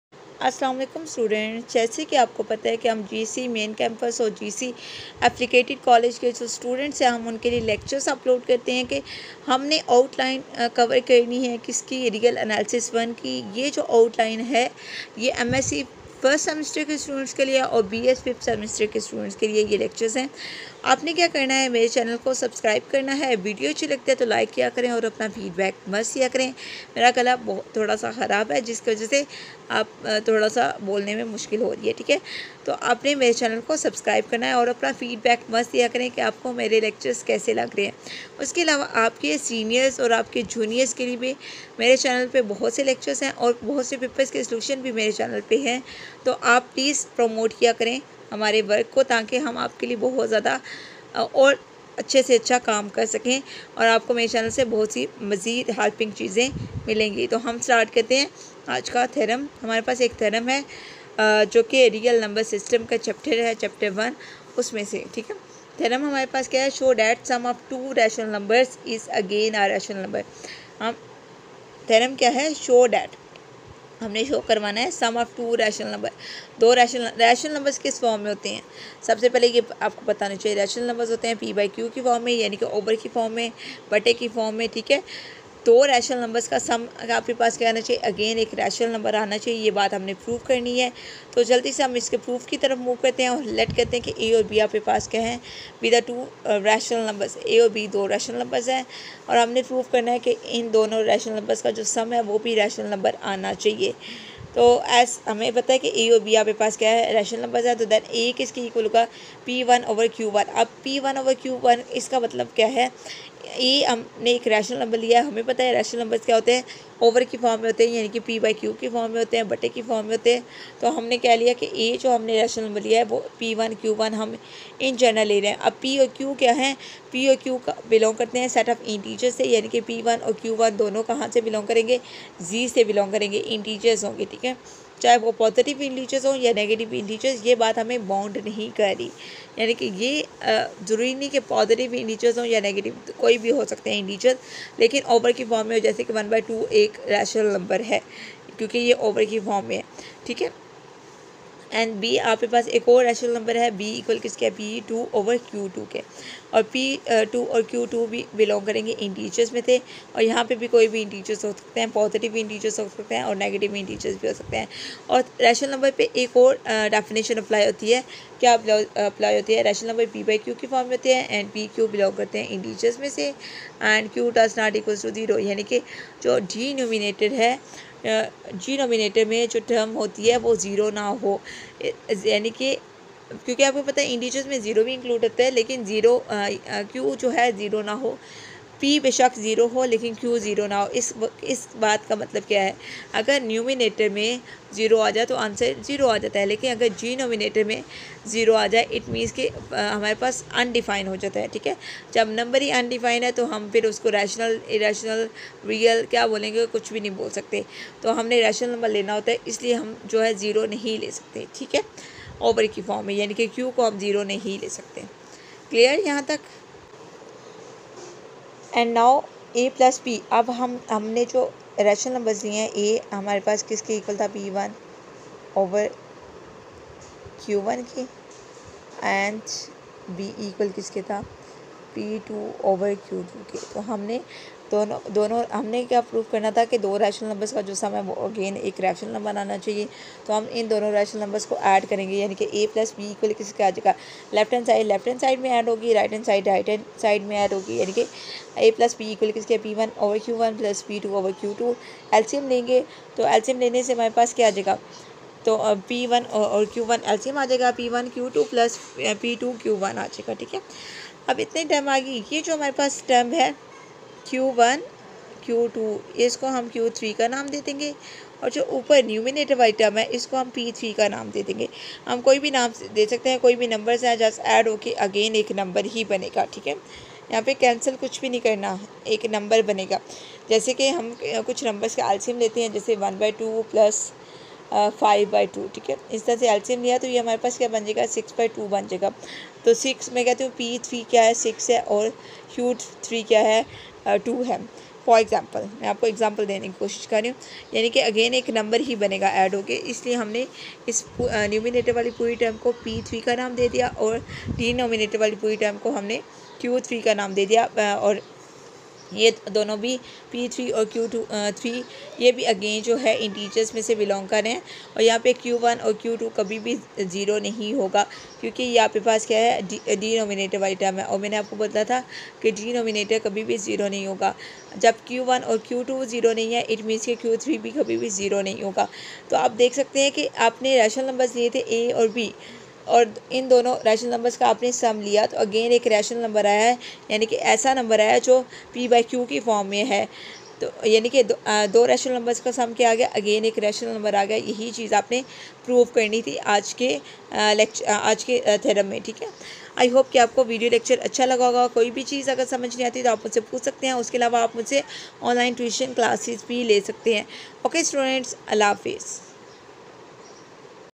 Show that sum of two rational numbers is again a rational number. , Real Analysis -
today I explain theorem
show that sum of two rational numbers is again a rational number .